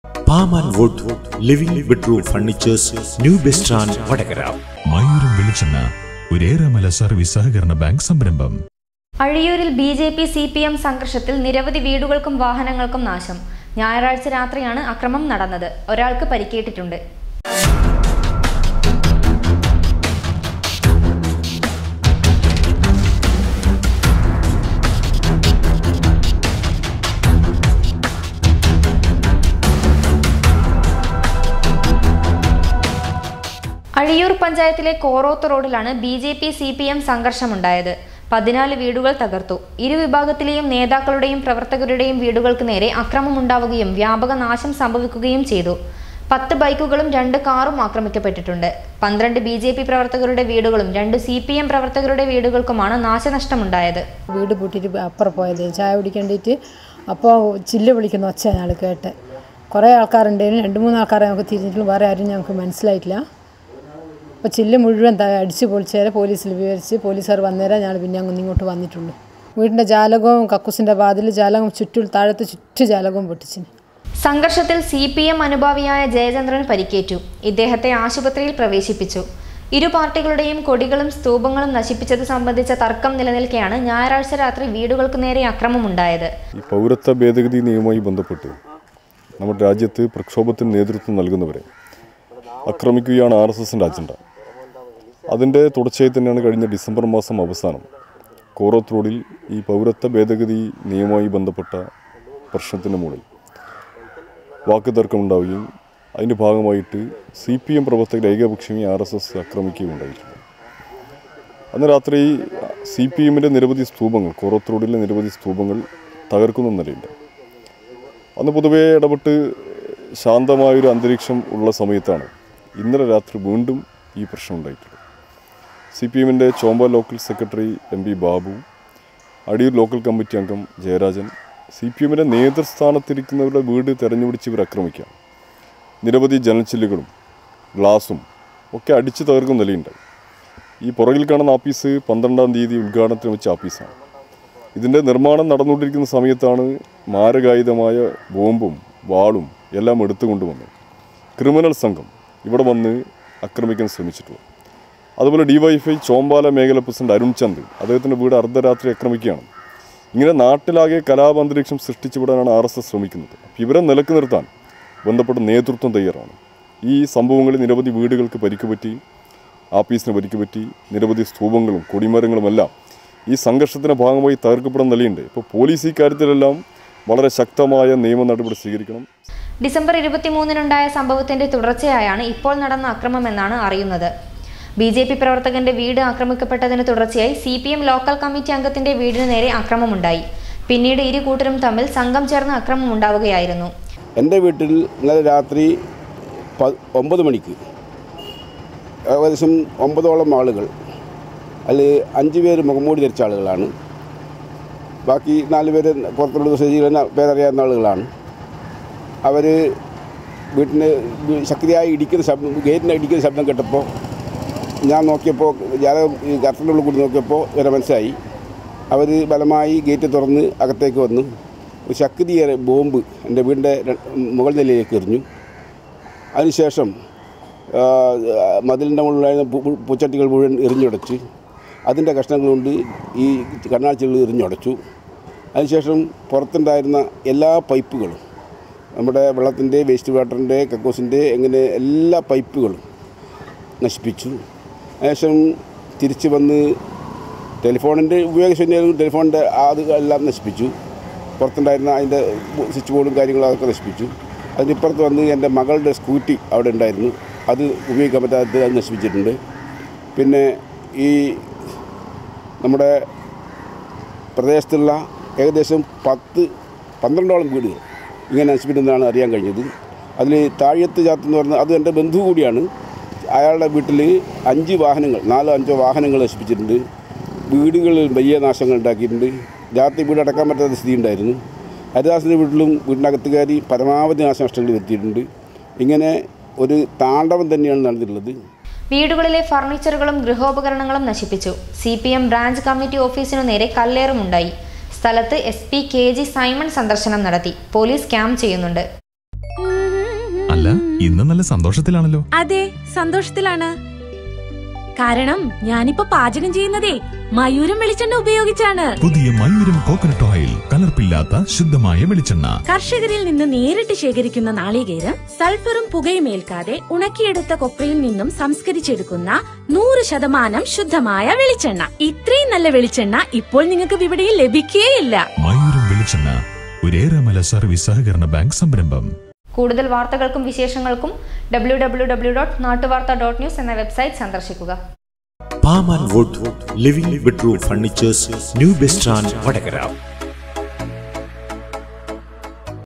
അഴിയൂരില്‍ ബിജെപി - സിപിഎം സംഘര്‍ഷം
അഴിയൂരില്‍ ബിജെപി-സിപിഎം സംഘര്‍ഷത്തില്‍ നിരവധി വീടുകള്‍ക്കും വാഹനങ്ങള്‍ക്കും നാശം. ഞായറാഴ്ച രാത്രിയാണ് അക്രമം നടന്നത്. ഒരാള്‍ക്ക് പരിക്കേറ്റിട്ടുണ്ട്.
അഴിയൂര്‍ പഞ്ചായത്തിലെ കോറോത്ത് റോഡിലാണ് ബിജെപി-സിപിഎം സംഘര്‍ഷമുണ്ടായത്. 14 വീടുകള്‍ തകര്‍ത്തു. ഇരു വിഭാഗത്തിലെയും നേതാക്കളുടെയും പ്രവര്‍ത്തകരുടെയും വീടുകള്‍ക്കു നേരെ അക്രമമുണ്ടാവകയും വ്യാപകനാശം സംഭവിക്കുകയും ചെയ്തു.പത്ത് ബൈക്കുകളും രണ്ട് കാറും ആക്രമിക്കപ്പെട്ടിട്ടുണ്ട്.  പന്ത്രണ്ട് ബിജെപി പ്രവര്‍ത്തകരുടെ വീടുകളും രണ്ടു സി.പി.എം പ്രവര്‍ത്തകരുടെ വീട്ടുകള്‍ക്കുമാണ് നാശനഷ്ടമുണ്ടായത്.
സംഘര്‍ഷത്തില്‍ സി.പി.എം അനുഭാവിയായ ജയചന്ദ്രനു പരിക്കേറ്റു. ഇദ്ദേഹത്തെ ആശുപത്രിയില്‍ പ്രവേശിപ്പിച്ചു. ഇരുപാര്‍ട്ടികളുടെയും കൊടികളും സ്തൂപങ്ങളും നശിപ്പിച്ചതു സംബന്ധിച്ച തര്‍ക്കം നിലനില്‍ക്കെയാണ് ഞായറാഴ്ച രാത്രി വീടുകള്‍ക്കു നേരെ അക്രമമുണ്ടായത്..ഡിസംബര്‍ ഇരുപത്തിമൂന്നിനുണ്ടായ സംഭവത്തിന്റെ തുടര്‍ച്ചയായാണ് ഇപ്പോള്‍ നടന്ന അക്രമമെന്നാണ് അറിയുന്നത്.
ബിജെപി പ്രവര്‍ത്തകന്റെ വീട് ആക്രമിക്കപ്പെട്ടതിനു തുടര്‍ച്ചയായി സിപിഎം ലോക്കല്‍ കമ്മിറ്റി അംഗത്തിന്റെ വീടിനു നേരെ അക്രമമുണ്ടായി.. പിന്നീട് ഇരുകൂട്ടരും തമ്മില്‍ സംഘം ചേര്‍ന്ന അക്രമമുണ്ടാവുകയായിരുന്നു. വീടുകളിലെ ഫര്‍ണിച്ചറുകളും ഗൃഹോപകരണങ്ങളും നശിപ്പിച്ചു. സിപിഎം ബ്രാഞ്ച് കമ്മിറ്റി ഓഫീസിനു നേരെ കല്ലേറുമുണ്ടായി. സ്ഥലത്ത് എസ്പി കെ.ജി. സൈമണ്‍ സന്ദര്‍ശനം നടത്തി.  പോലീസ് ക്യാമ്പ് ചെയ്യുന്നുണ്ട്.